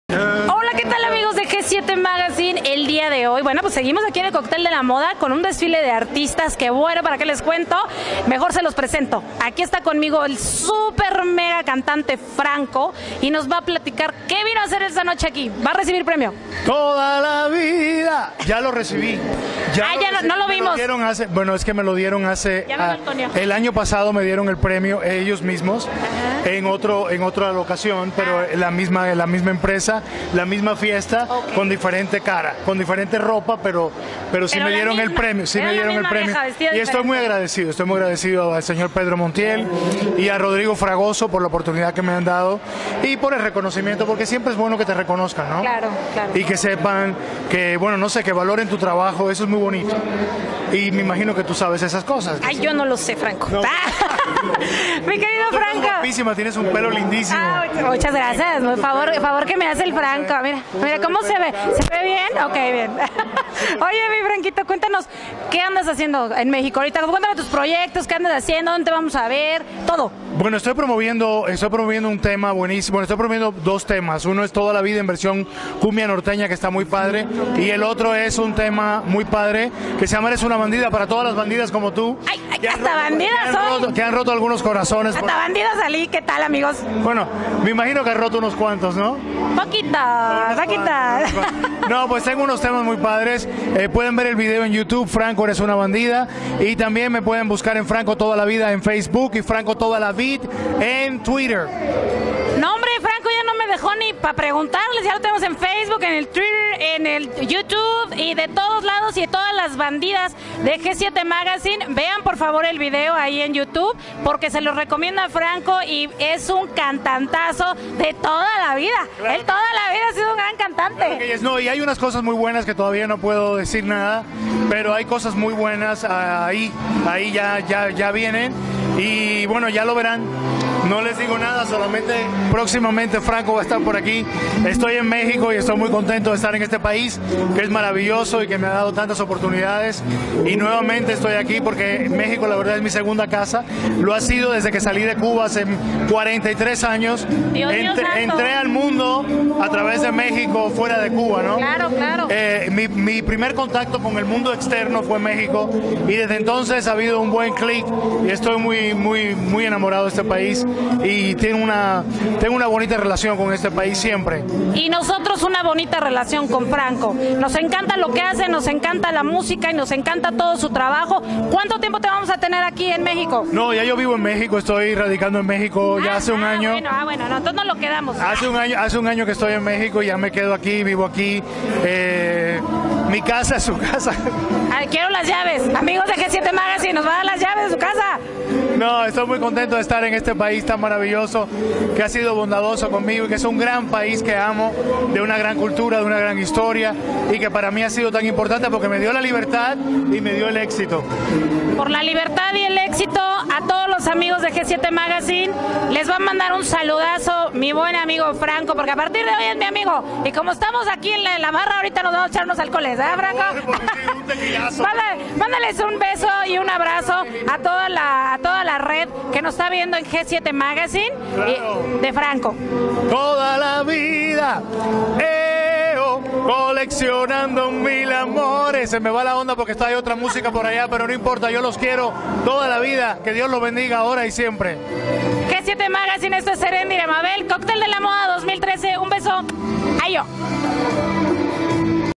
G7 Magazine:Franco: Entrevista y Fotos
Franco de toda la vida nos comparte un poco de su trayectoria como cantante.